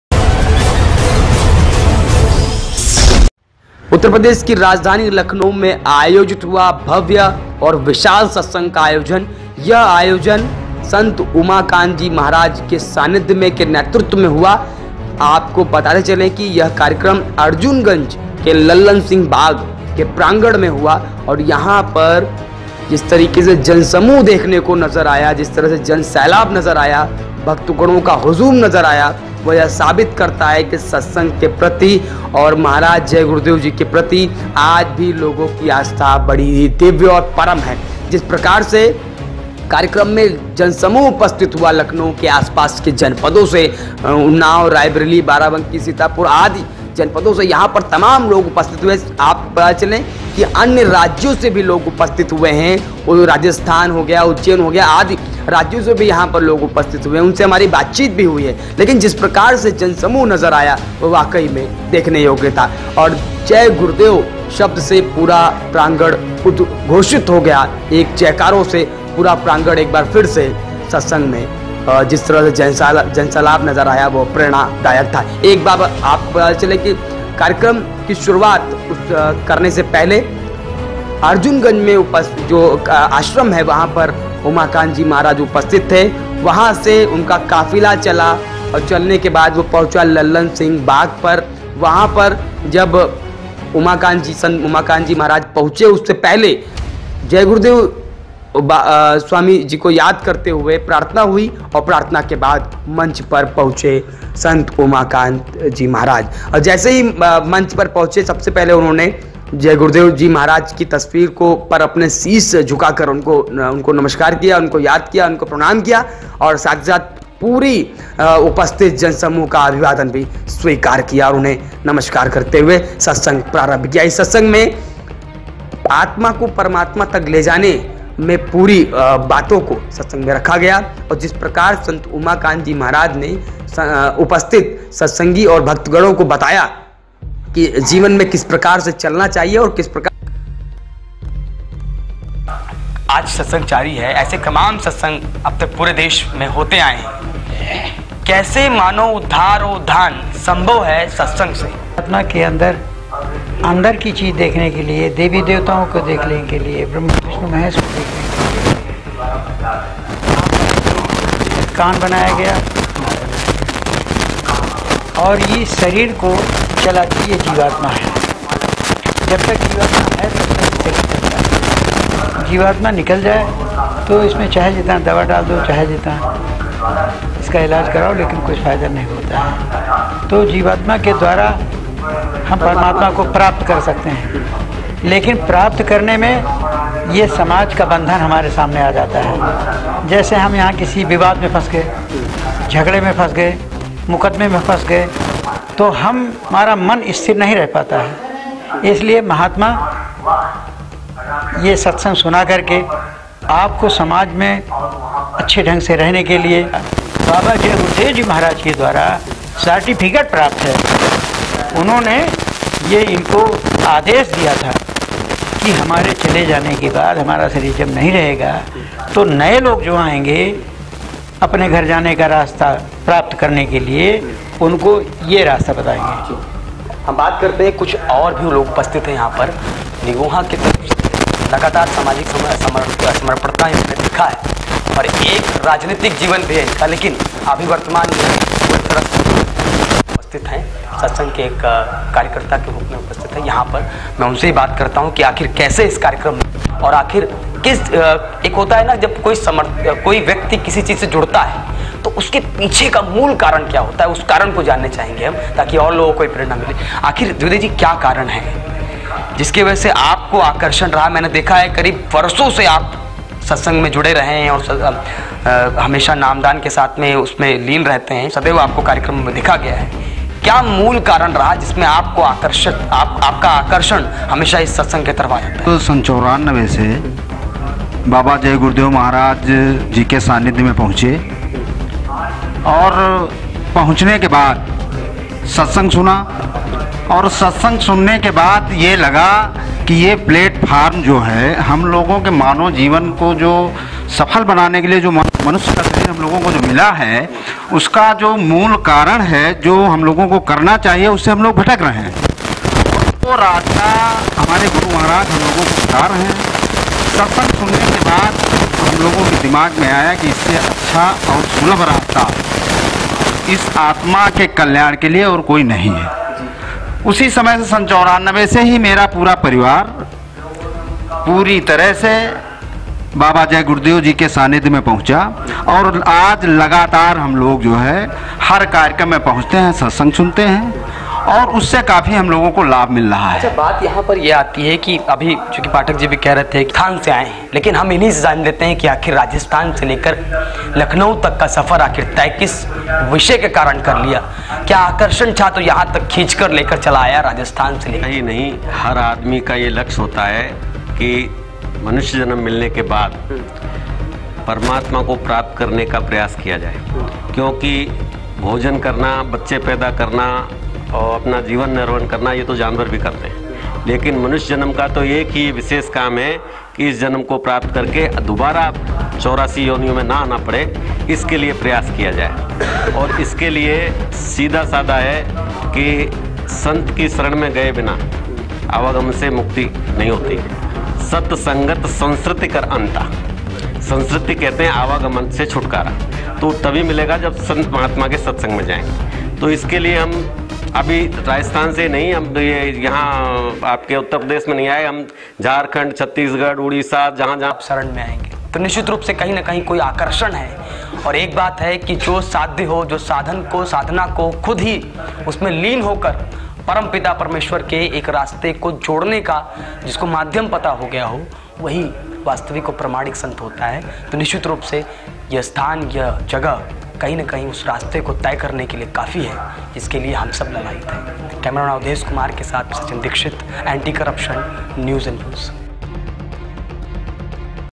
राजधानी लखनऊ में हुआ सत्संग का भव्य आयोजन,हर तरफ गूंजा जय गुरु देव वाक्य,बताया गया मुक्ति का रास्ता
लखनऊ के अर्जुनगंज के लल्लन सिंह के बाग में आयोजित हुआ संत उमाकांत जी महाराज के नेतृत्व में सत्संग का कार्यक्रम,जिसमे हजारो भक्तगणों का आगमन हुआ आस पास के दर्जनों जनपदों से पधारे भक्तगण